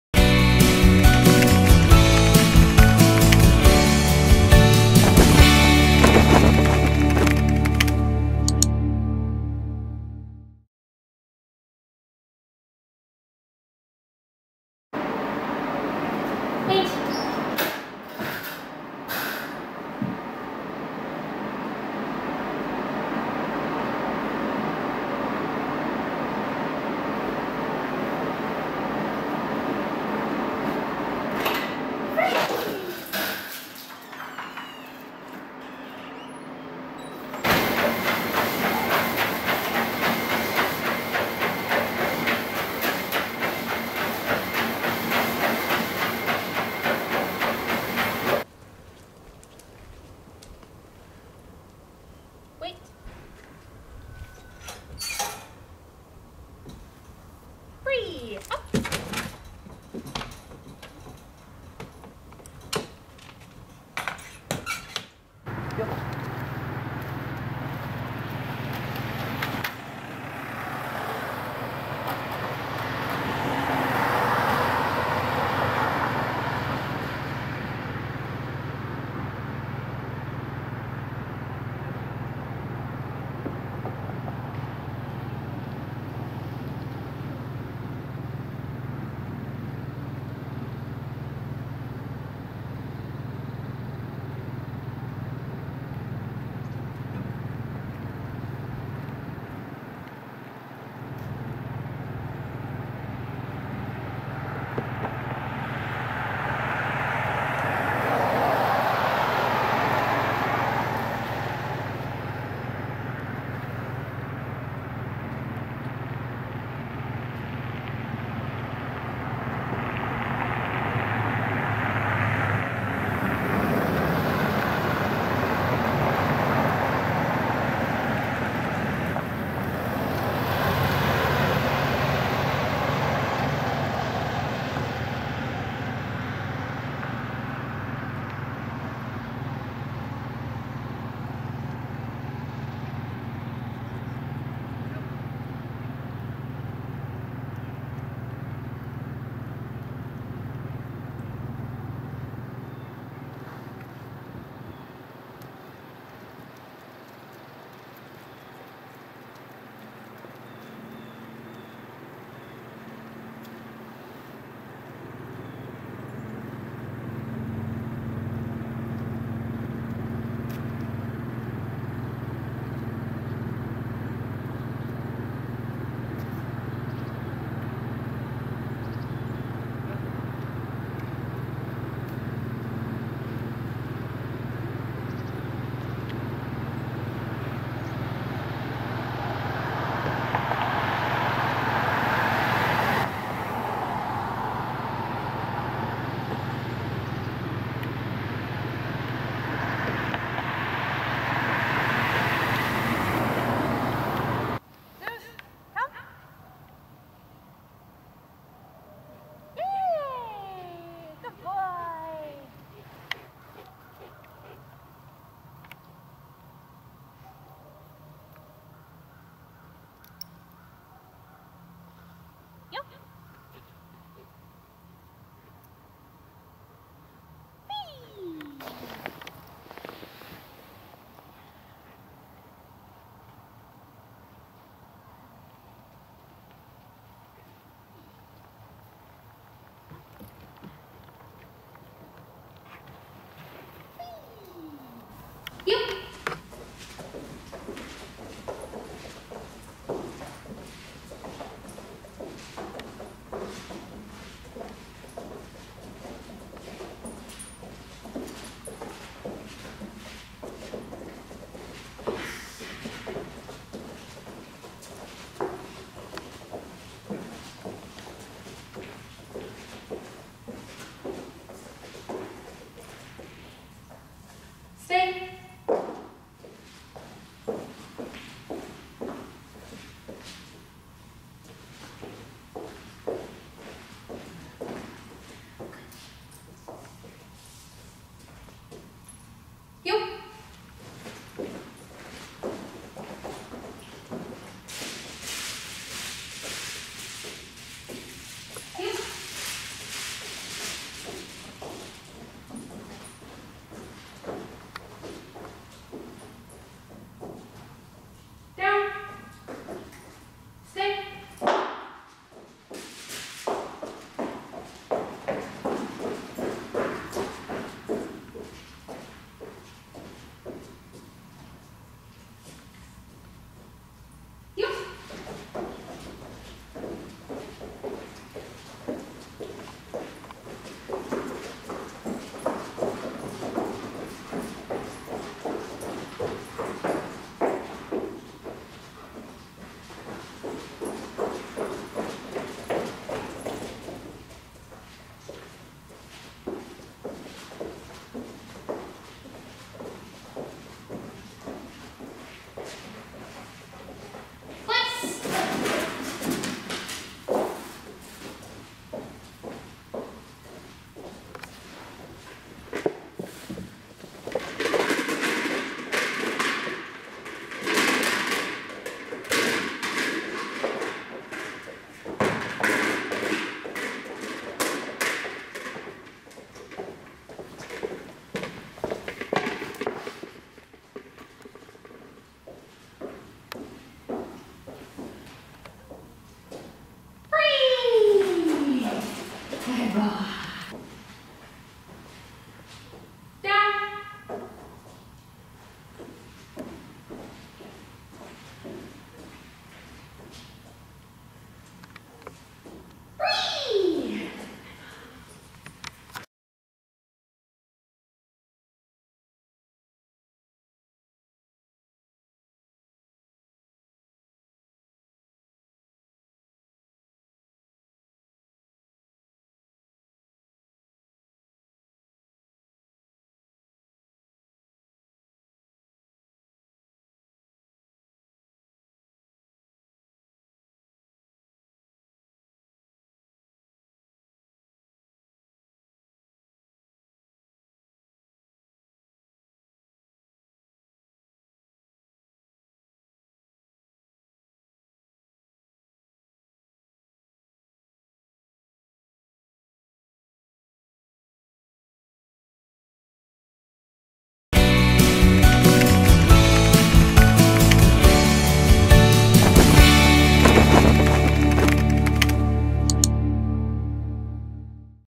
Zeus (German Shepherd Dog) Boot Camp Dog Training Video Demonstration
Zeus (German Shepherd Dog) graduated from the dog training boot camp at Neuman K-9 Academy. This program included obedience commands to sit, stay, heel or walk on a loose leash, come when called, proper etiquette, no jumping up, meeting and greeting people under control, and running on a treadmill.

Our dog training camp provides programs for German Shepherd Dog's such as boot camp, obedience training, and puppy camp.